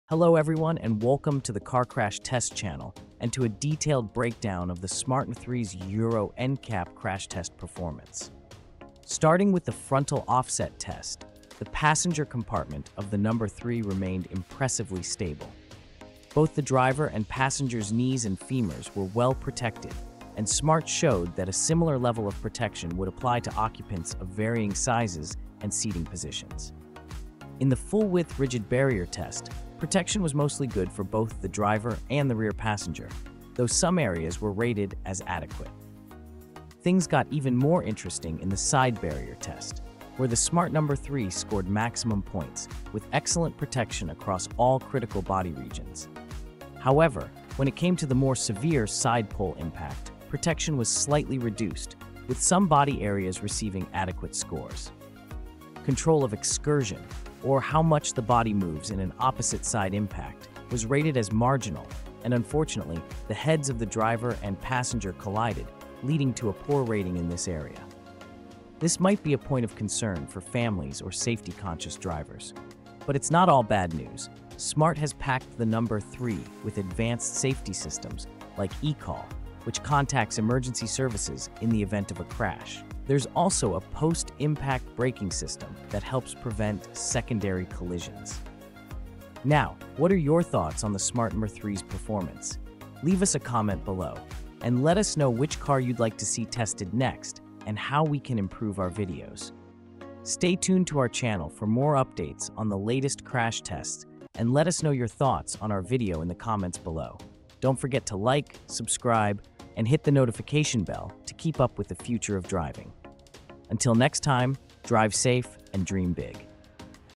2024 Smart #3 Euro NCAP Crash Test Results | In-Depth Safety Breakdown
Smart #3 Euro NCAP Crash Test Results | In-Depth Safety Breakdown*

How does the compact and stylish Smart #3 perform in crash tests? In this video, we analyze its Euro NCAP crash test results, exploring how the vehicle protects occupants during frontal, side, and rear-end collisions. Curious about how it handles extreme impact scenarios?